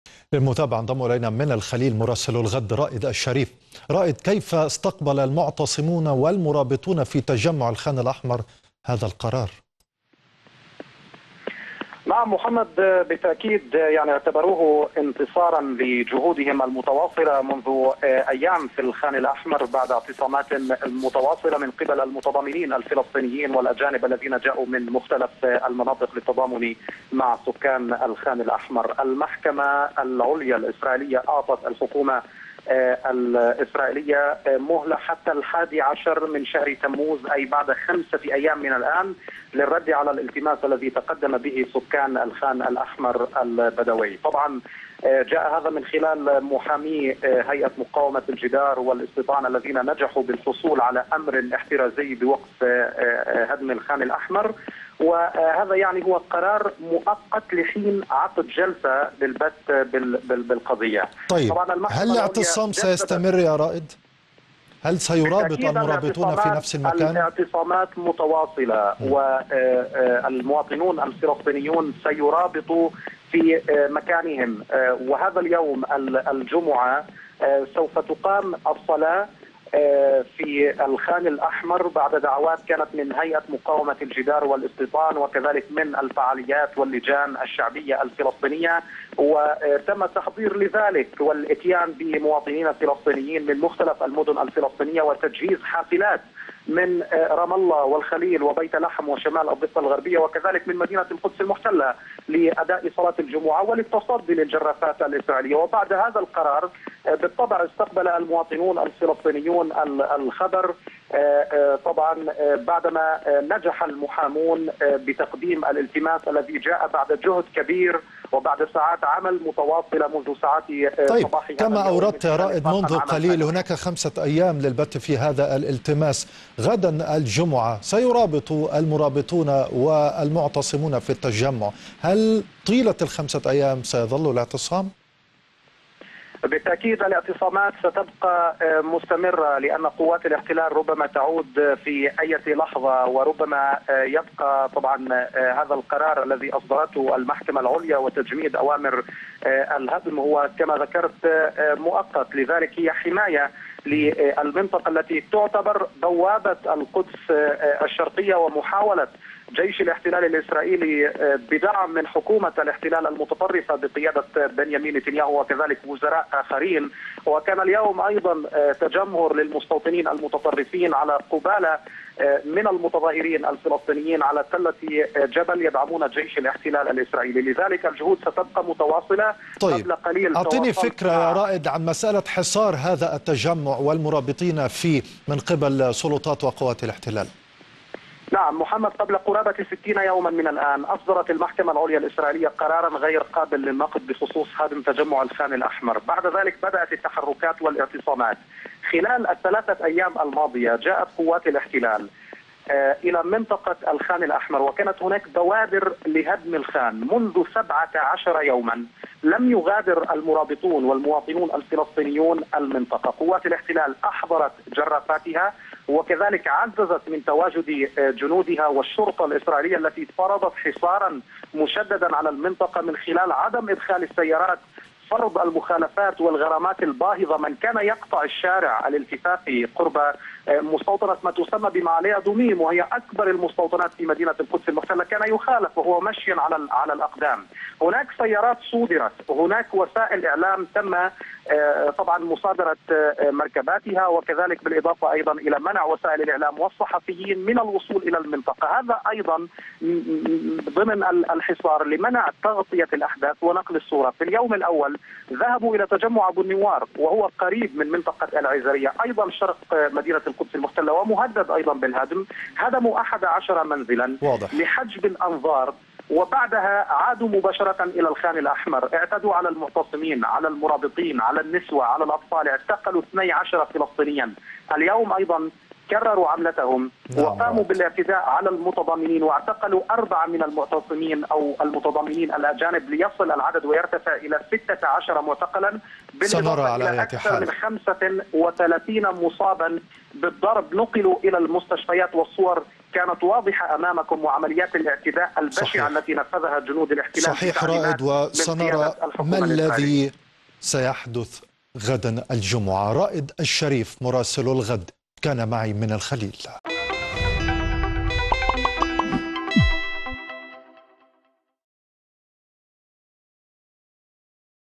مراسل الغد: استمرار اعتصام الفلسطينيين بالخان الأحمر والقرار الإسرائيلي بتجميد الهدم مؤقت
أصدرت المحكمة العليا الإسرائيلية، اليوم الخميس، قرارًا يقضي بتجميد مؤقتا لهدم تجمع الخان الأحمر بين القدس وأريحا، بشكل مؤقت وشرع الاحتلال الإسرائيلي بالأمس في عمليات هدم خان الأحمر باستخدام الجرَّاف، مستعينًا بالقوات الامنية لقمع المواطنين الذين يحاولون التصدي لعمليات الهدم... المزيد من التفاصيل يتحدث رائد الشريف مراسل الغد من الخليل، مع محمد عبد الله، عبر الفقرة الإخبارية.